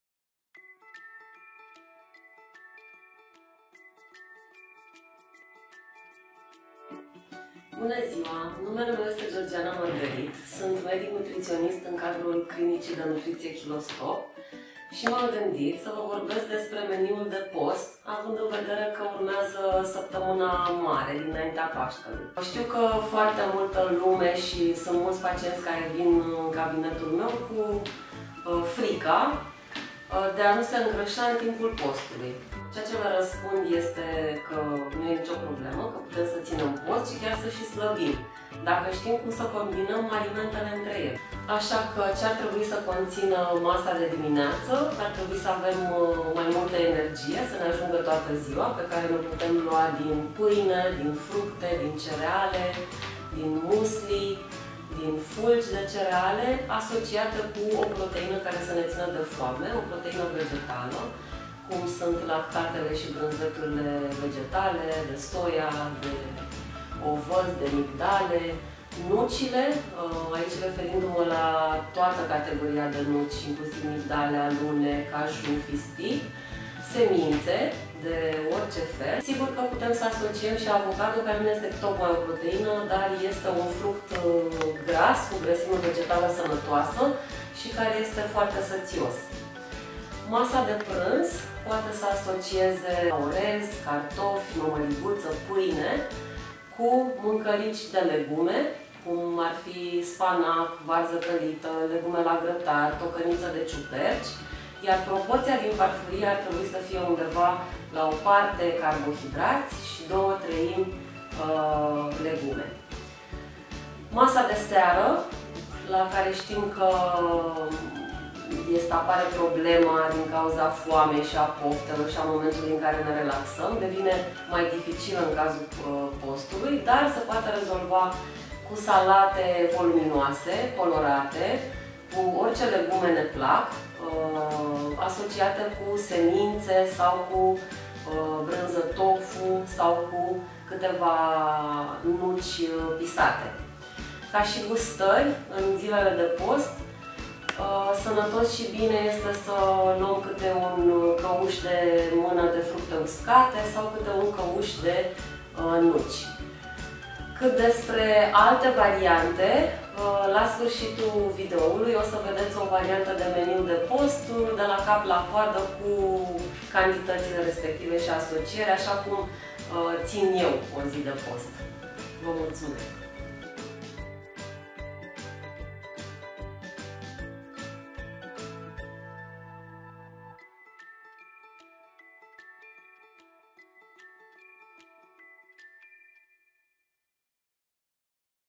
Cum să ții o dietă de post
Pentru că mulți dintre noi țin post în săptămâna dinaintea Paștelui, dr. Georgiana Mărgărit ne dezvăluie astăzi principiile de care să ținem cont ca să nu ne îngrășăm.
Urmărește filmul până la final, unde vei găsi și dieta de post de 1 zi preferată de Dr. Georgiana Mărgărit.